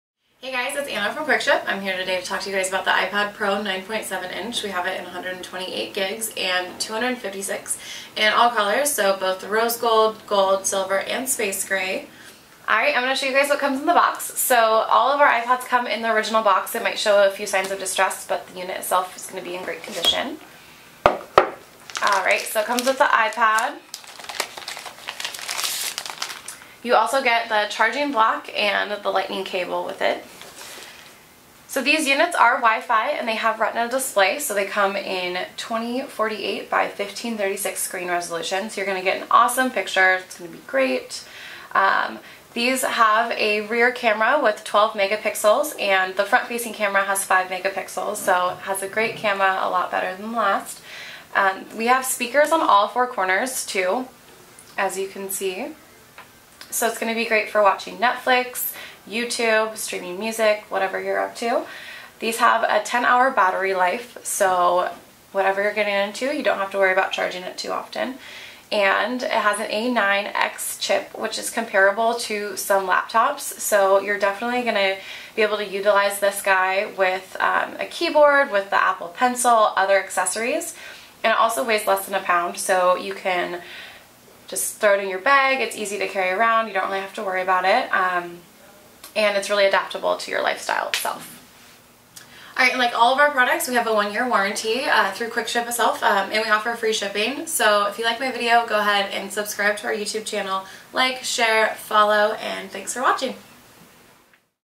Show Me What I Get- iPad Pro 9.7"
The Apple iPad Pro Tablet sports a 9.7-inch retina display with a resolution of 2048x1536 pixels and True Tone display. Powered by the A9X chip, and M9 motion coprocessor, it provides high speed and efficiency. This Wi-Fi model has a storage capacity of 128 GB, and runs on the IOS9 operating system. It features a 12 MP iSight camera, allowing you to capture sharp and detailed photographs. In addition, this tablet supports the Smart Keyboard and the Apple Pencil, and is available in all colors.

Curious about our iPads?